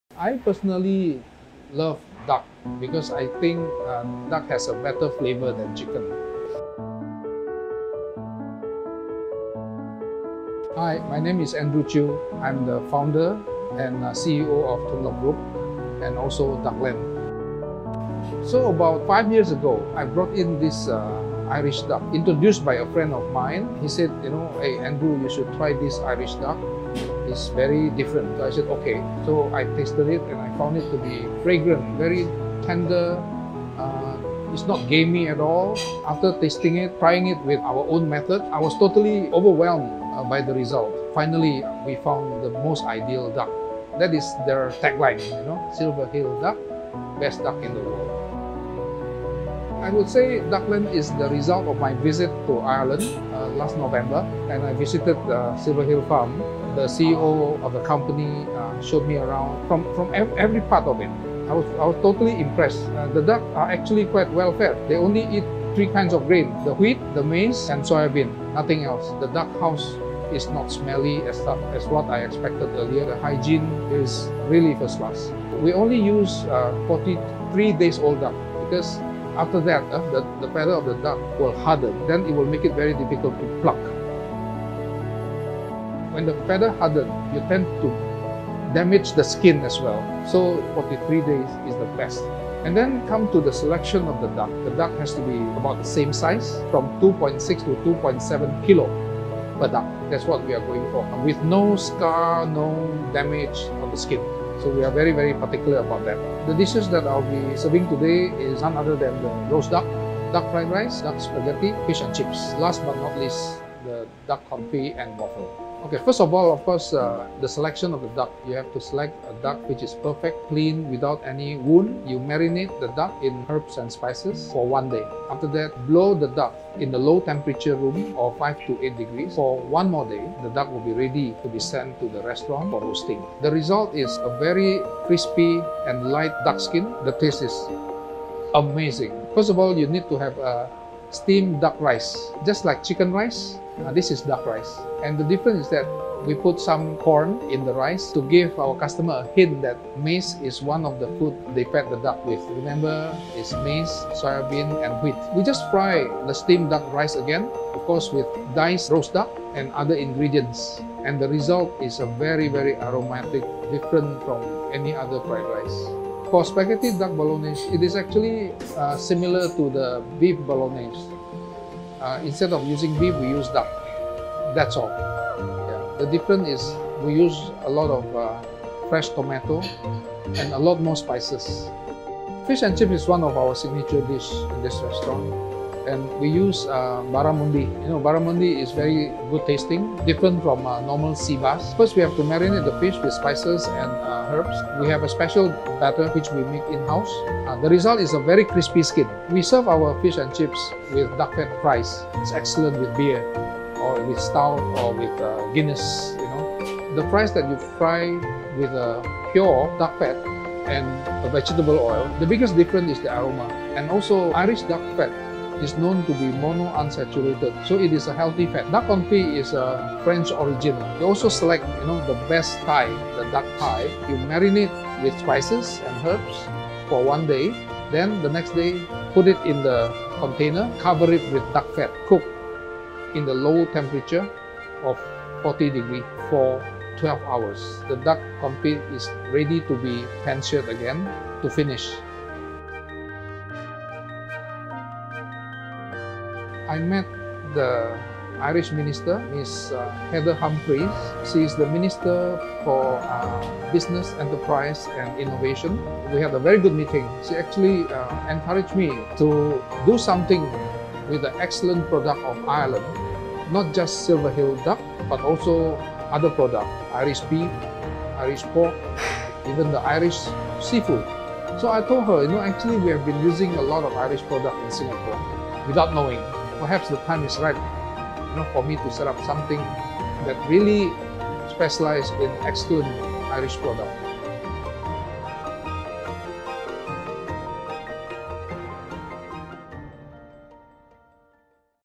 The Best Duck in the World
Duckland uses Irish ducks from Silver Hill duck farm known as "The Best Duck In The World" for all of their duck dishes.